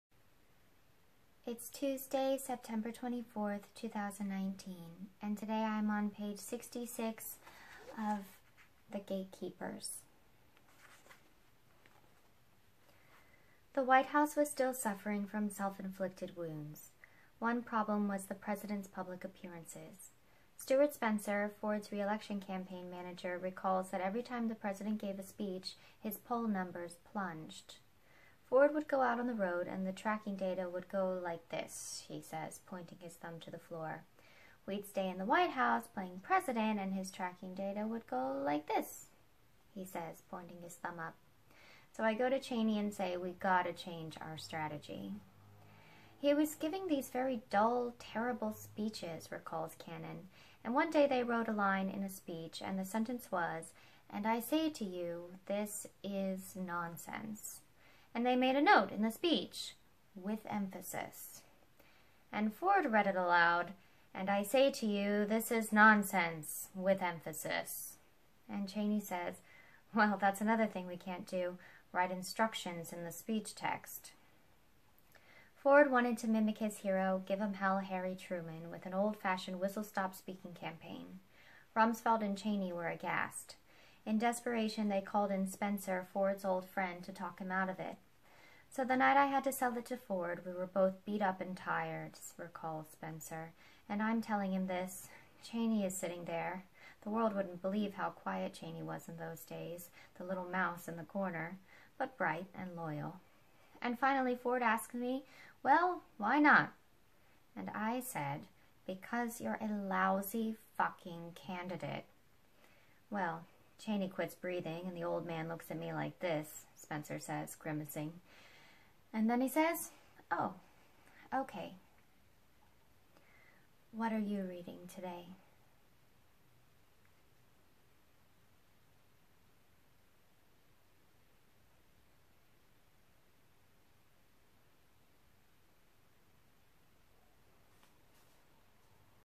2-Minute Tuesday Reads: The Gatekeepers
The Gatekeepers: How the White House Chiefs of Staff Define Every Presidency by Chris Whipple

Broadway Books, 2017, 2018

pg. 66 // Political Science, American Government, Executive Branch